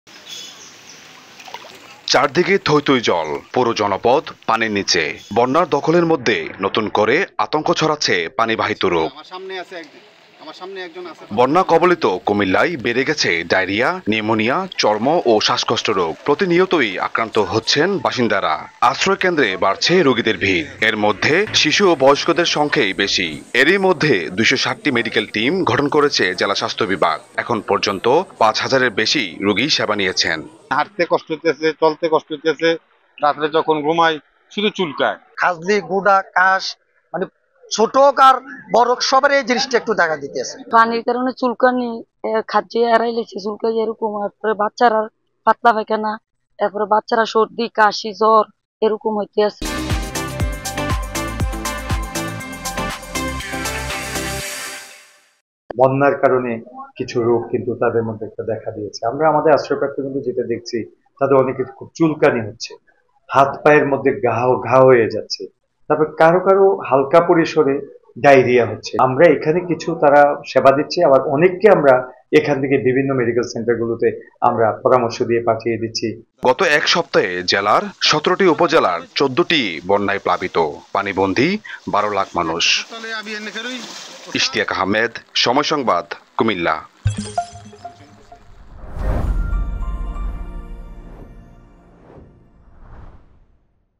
কুমিল্লায় বন্যায় মড়ার উপর খাঁড়ার ঘা | Flood Update | Sick | Comilla | Somoy TV
কুমিল্লায় বন্যার পানি কিছুটা কমলেও পানিবন্ধি মানুষের মাঝে দেখা দিয়েছে পানিবাহিত রোগ। প্রতিদিন মানুষ চিকিৎসার জন্য ভিড় করছেন স্বাস্থ্য কমপ্লেক্সগুলোতে। চিকিৎসা সেবার পাশাপাশি ওষুধ সহায়তা দিচ্ছে আশ্রয় কেন্দ্রের মেডিকেল টিম।